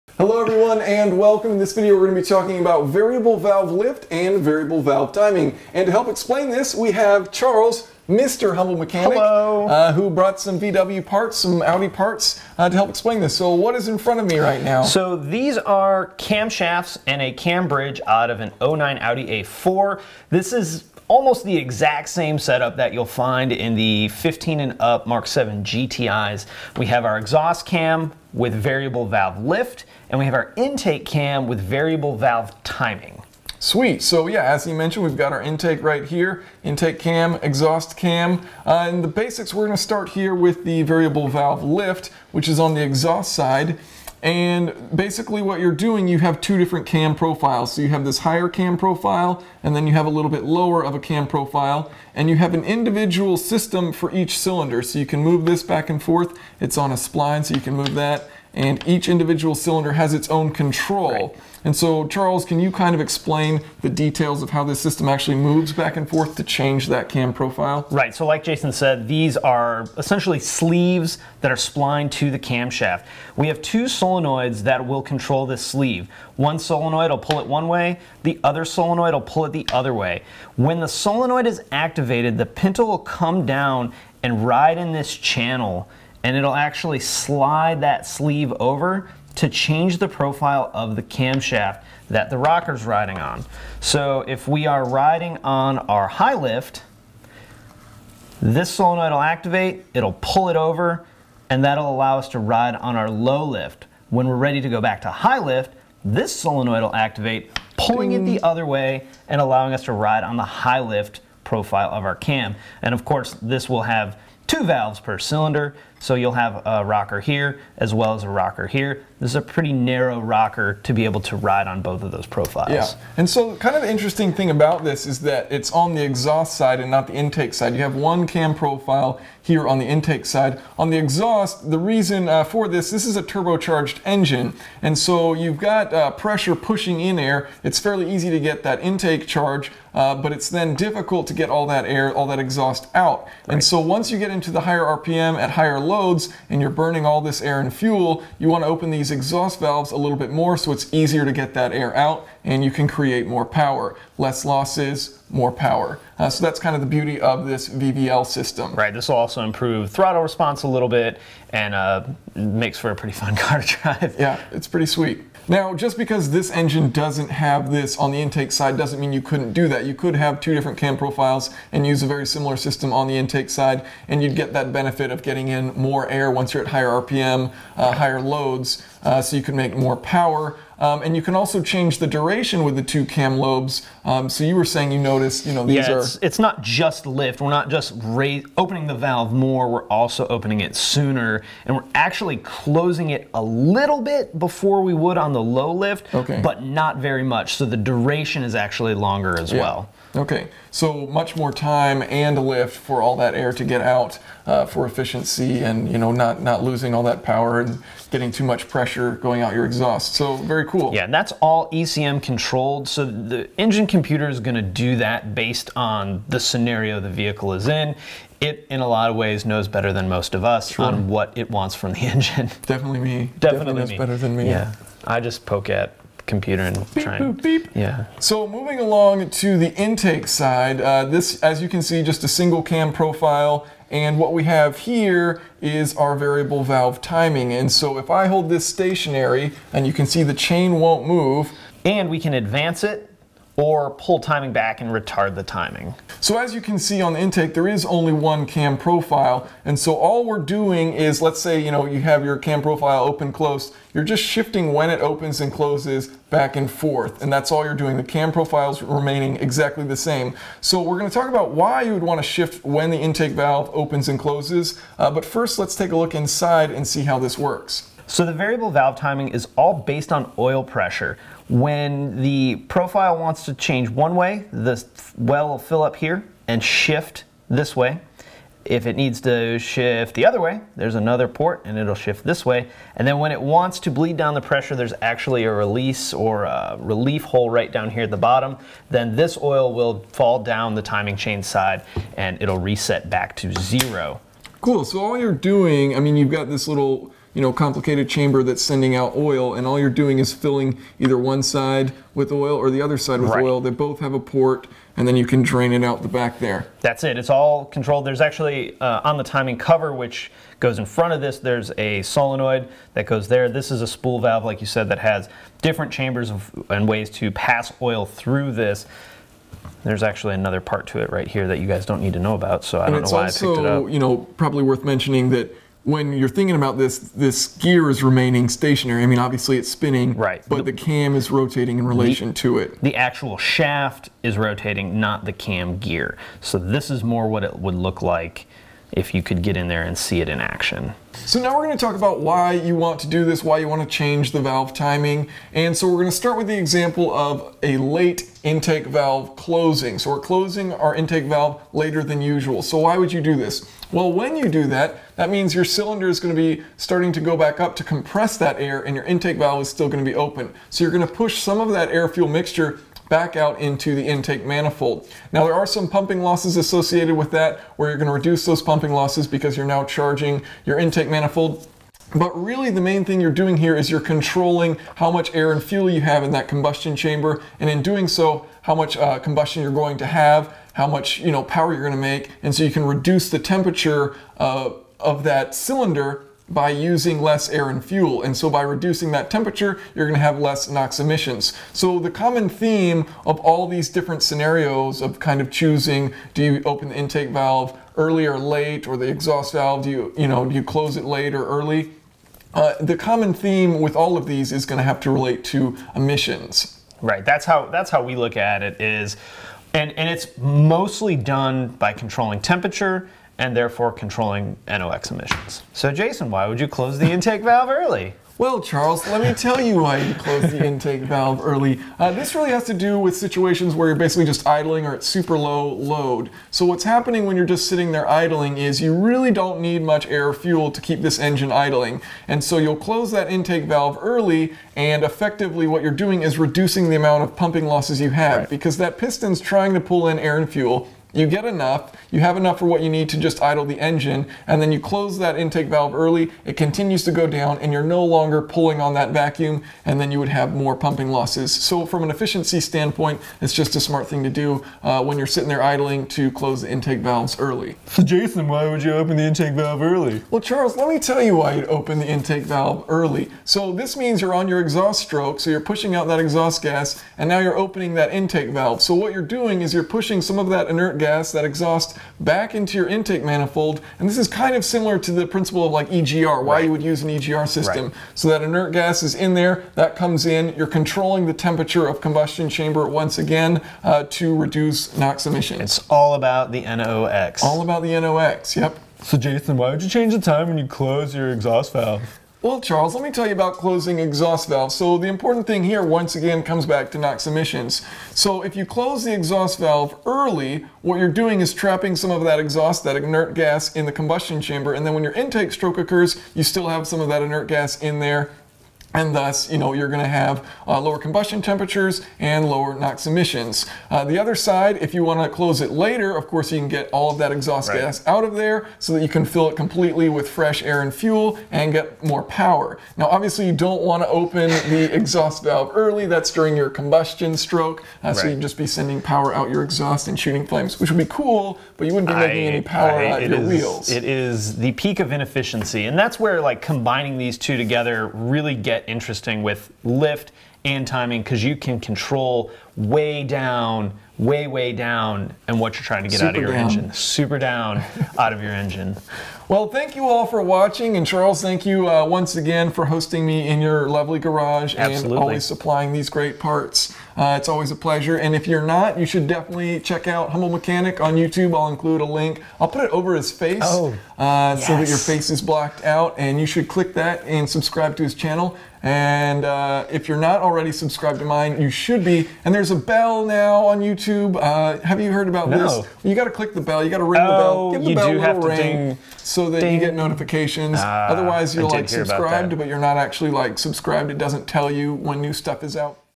Variable Valve Lift vs Variable Valve Timing - VVL vs VVT
What is variable valve lift? What is variable valve timing? We're going to look at the valvtrain from a 2009 Audi 2.0L I4 Turbo engine to check out the differences. Variable valve lift is used to enhance performance depending on the RPM and load, while variable valve timing is typically used for emissions control (lowering combustion temperatures and thus reducing NOx), but also has performance and efficiency benefits as well.